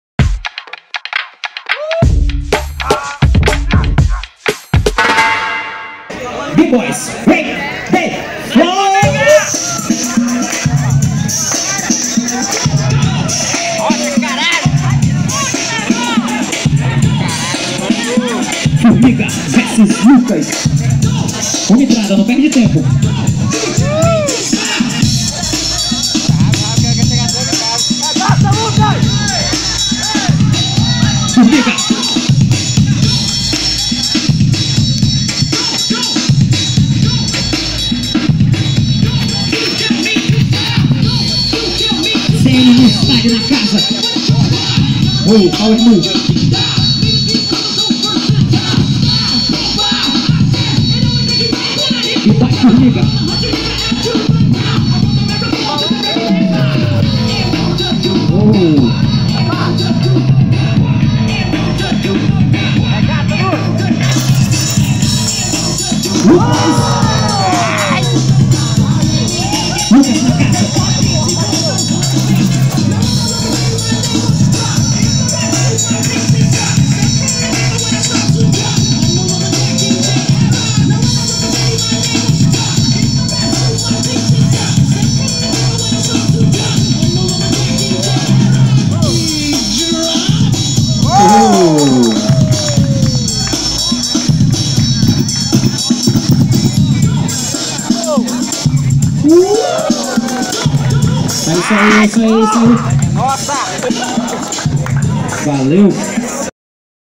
Break the Floor América Latina 2017 I Lucas vs Formiga
Evento realizado na cidade de Manaus-AM, uma seletiva para participar em CANNES França na grande final do BREAK THE FLOOR 2018.

Assine nossa notificação e ajude a gente a crescer!
Compartilhe no seu FACEBOOK com os seus amigos :)